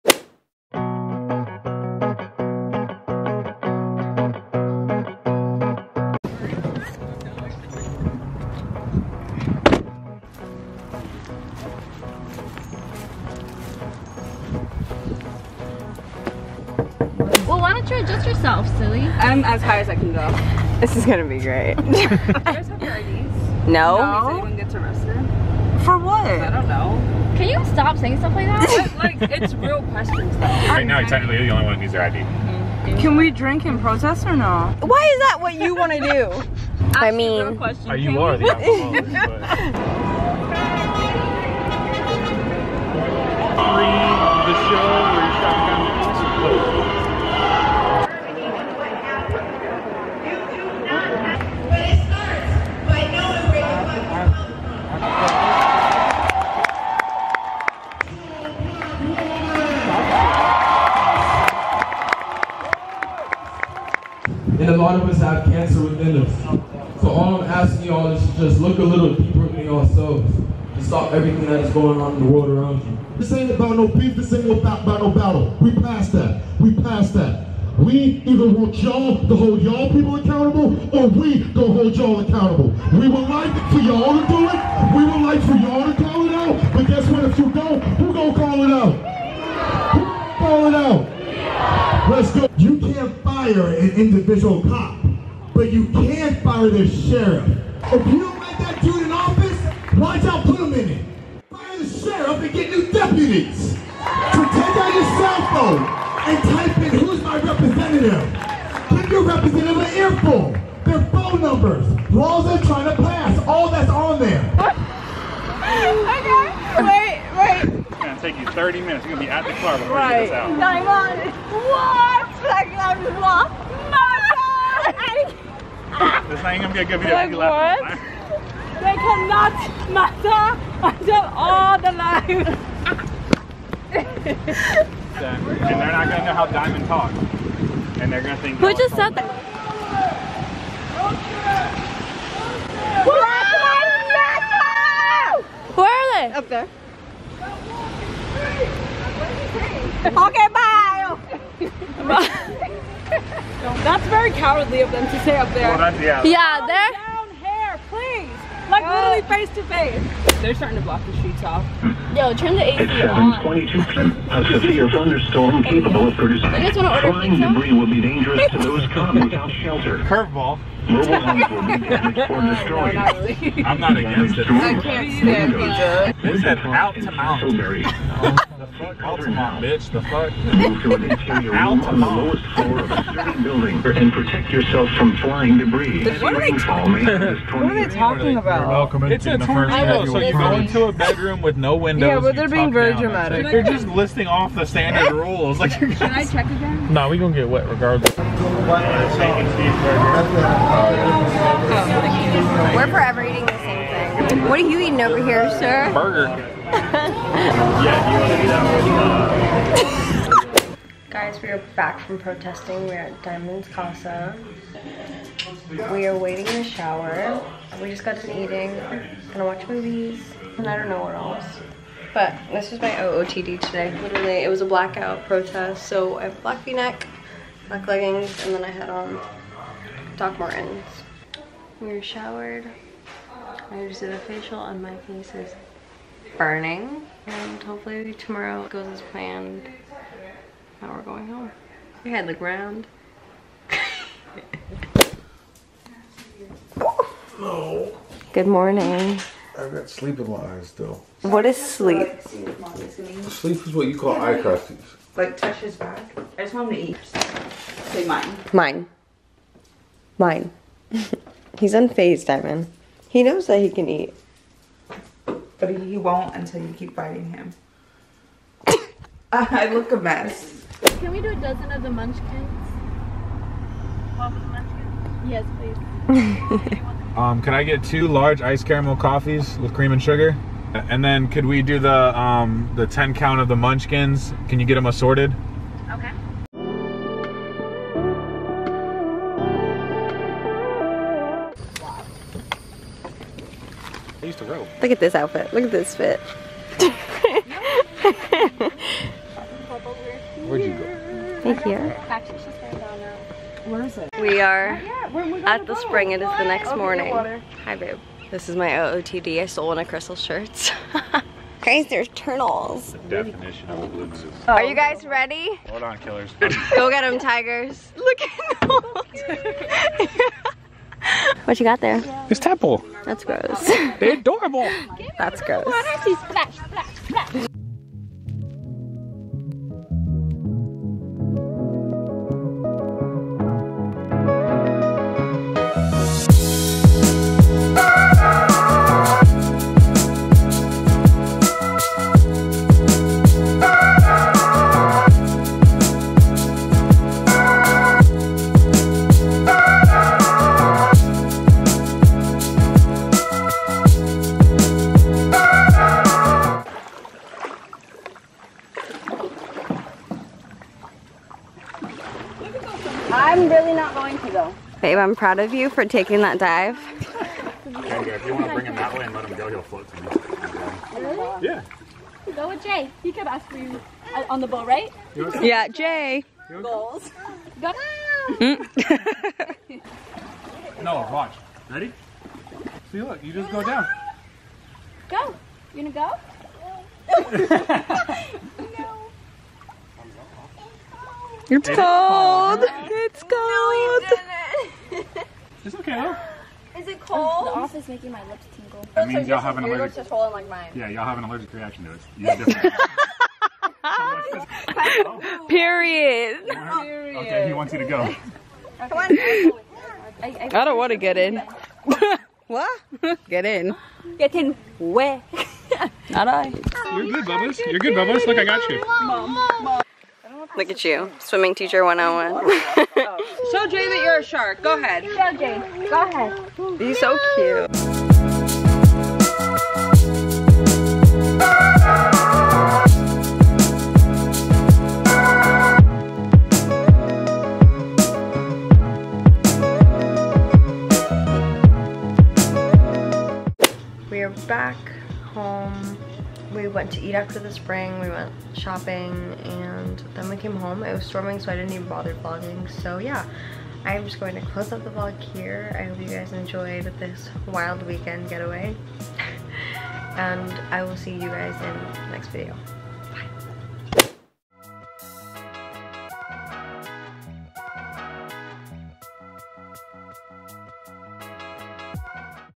WE WENT TO A BLM PROTEST !!!
My little family and I decided to support our people and get away for the weekend, enjoy the vlog!